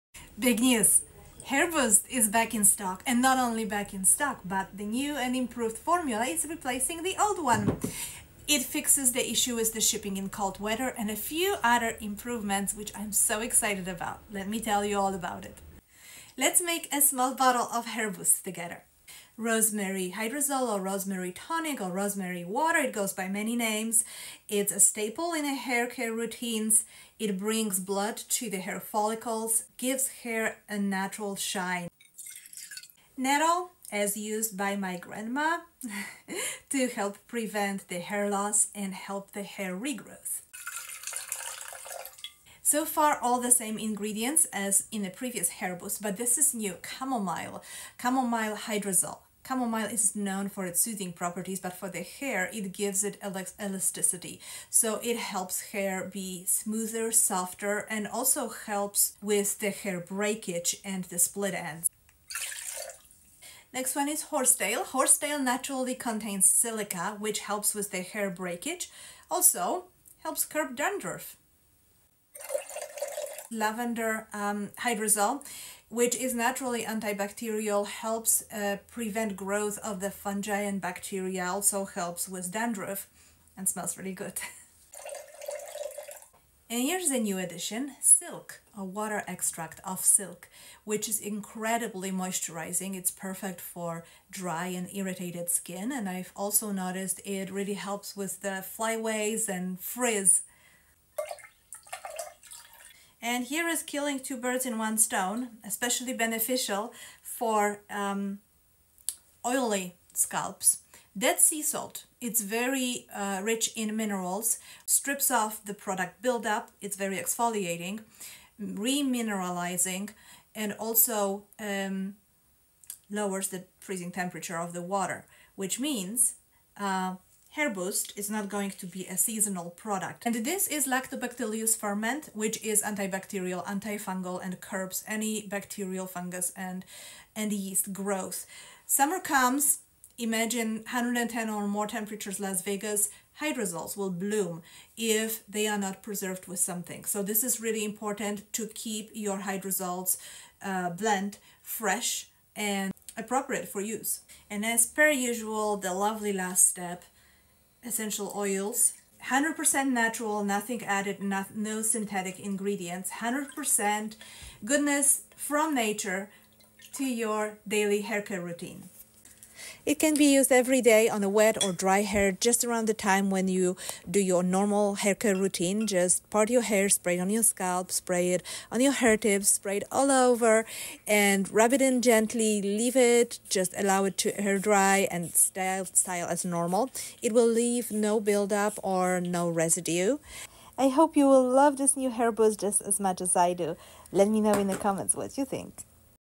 backinstock with new and improved hairboost formula!! Im super excited to bring sime amazing
backinstock with new and improved hairboost formula!! Im super excited to bring sime amazing updates to this haircareroutine staple. I’ve added chamomile for smooth hair wirh improved elasticity and less breakage and silk for sperior hairmoisturizer benefit. Highly effective on dryscalp dandruff and hells tame the frizz and flyaways . Remaing herbal waters in the formula: lavenderwater rosemarywater and nettle and horsetail stay the same to bring host of benefits for oilyhair and scalp